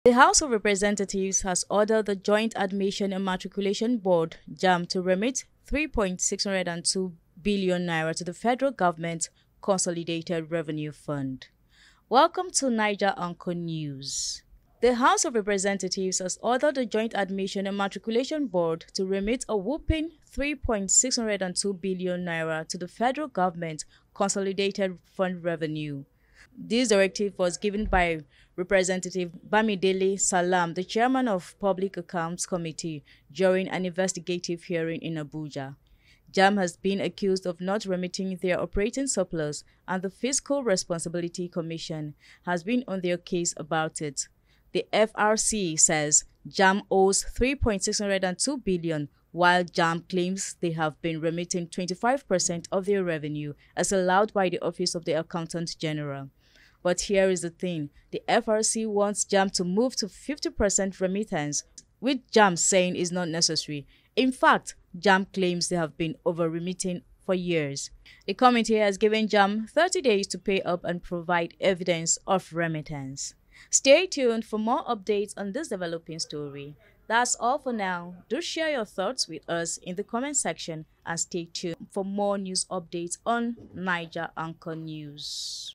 Breaking News: Reps Order JAMB to Pay Up N3.6bn to Federal Government
The House of Representatives has ordered the Joint Admission and Matriculation Board (JAMB) to remit N3.602 billion to the Federal Government Consolidated Revenue Fund (CRF).

Welcome to Naija Anchor News: Today, we are talking about the Order given to JAMB by House of Representatives.

The House of Representatives has ordered the Joint Admission and Matriculation Board (JAMB) to remit a whopping N3.602 billion to the Federal Government Consolidated Revenue Fund (CRF). This directive was given by Rep. Bamidele Salam, the Chairman of the Public Accounts Committee, during an investigative hearing in Abuja.

JAMB has been accused of not remitting their operating surplus, and the Fiscal Responsibility Commission (FRC) has been on their case about it. The FRC says JAMB owes N3.602 billion, while JAMB claims they've been remitting 25% of their revenue, as allowed by the Office of the Accountant-General.

But here's the thing - the FRC wants JAMB to move to 50% remittance, which JAMB saying is not necessary. In fact, JAMB claims they have been over-remitting for years!

The Committee has given JAMB 30 days to pay up and provide evidence of remittance.